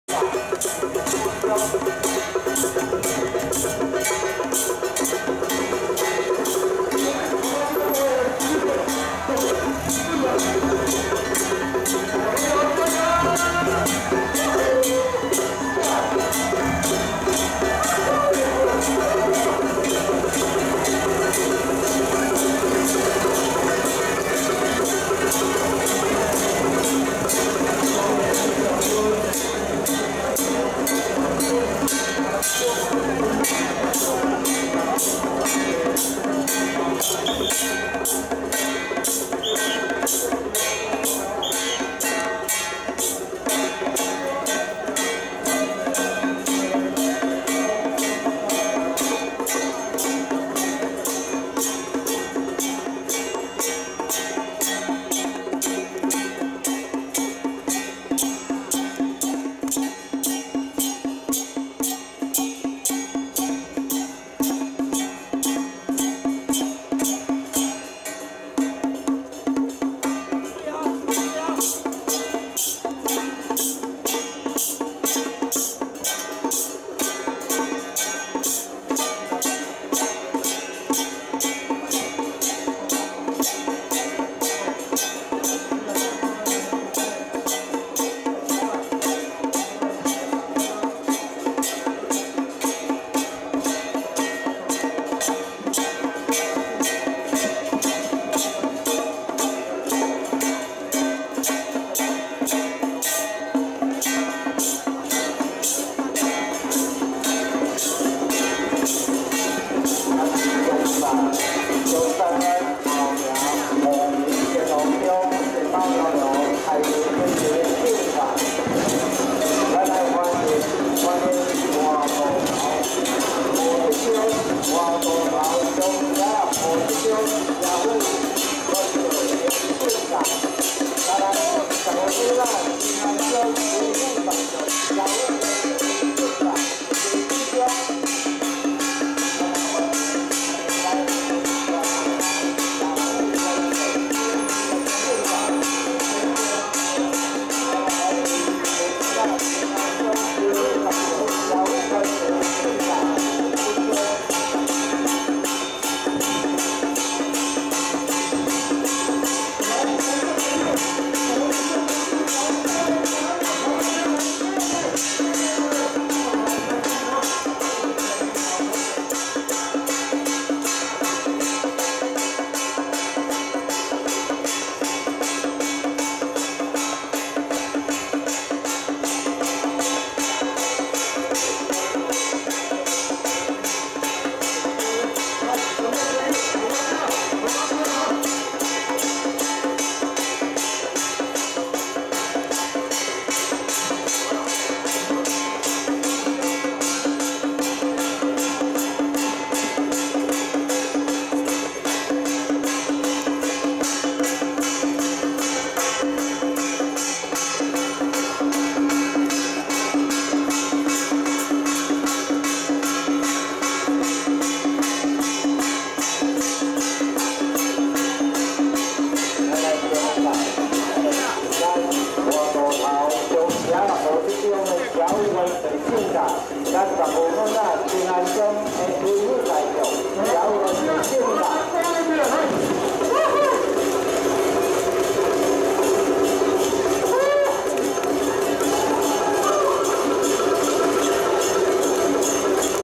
107.6.3西港慶安宮香科代天巡狩盧千歲遶境12【西港後營鳳儀仙鳳宮&什份塭清安宮－謝府元帥】
2018/6/3台南西港玉勅慶安宮戊戌香科代天巡狩盧千歲聖駕九十六村庄出巡遶境大典(西港香，尾日香)
↓ ↓ ↓更多相關影片 ↓ ↓ ↓